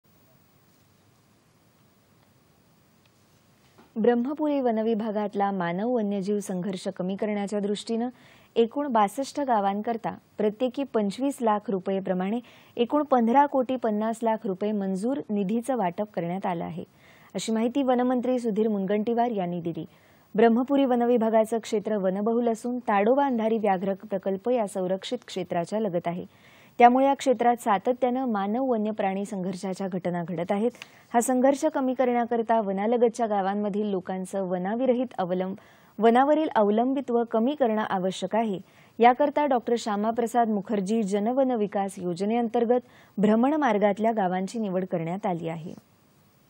६२ गावांकरिता १५ कोटी ५० लाख रूपये मंजूर निधीचं वाटप
दि.०३.१२.२०२२ :
 ब्रह्मपूरी वनविभागातला मानव-वन्यजीव संघर्ष कमी करण्याच्या दृष्टीनं एकूण ६२ गावांकरिता प्रत्येकी २५ लाख रूपये प्रमाणे एकूण १५ कोटी ५० लाख रूपये मंजूर निधीचं वाटप करण्यात आलं आहे, अशी माहिती वनमंत्री सुधीर मुनगंटीवार यांनी दिली. ब्रह्मपूरी वनविभागाचं क्षेत्र वनबहुल असून ताडोबा अंधारी व्याघ्र प्रकल्प या संरक्षित क्षेत्राच्या लगत आहे. त्यामुळे या क्षेत्रात सातत्यानं मानव-वन्यप्राणी संघर्षाच्या घटना घडत आहेत. हा संघर्ष कमी करण्याकरिता वनालगतच्या गावांमधील लोकांचे वनावरील अवलंबत्व कमी करणे आवश्यक आहे. याकरिता डॉ. श्यामाप्रसाद मुखर्जी जन-वन विकास योजनेअंतर्गत भ्रमण मार्गातल्या गावांची निवड करण्यात आली आहे.